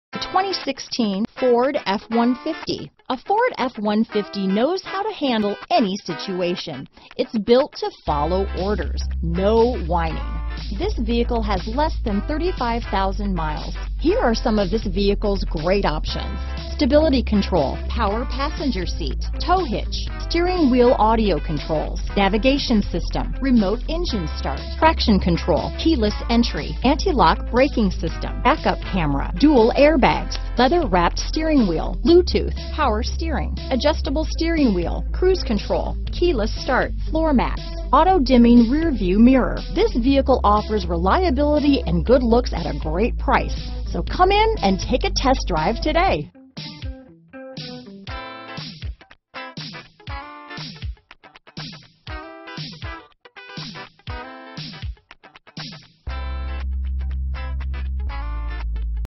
2016 Ford F-150 Atlanta, Marietta, Decatur, Johns Creek, Alpharetta, GA U4913
White Platinum Metallic Tri-Coat Used 2016 Ford F-150 available in Atlanta, Georgia at Hennessy Ford. Servicing the Marietta, Decatur, Johns Creek, Alpharetta, GA area.

Hennessy Ford
5675 Peachtree Industrial Blvd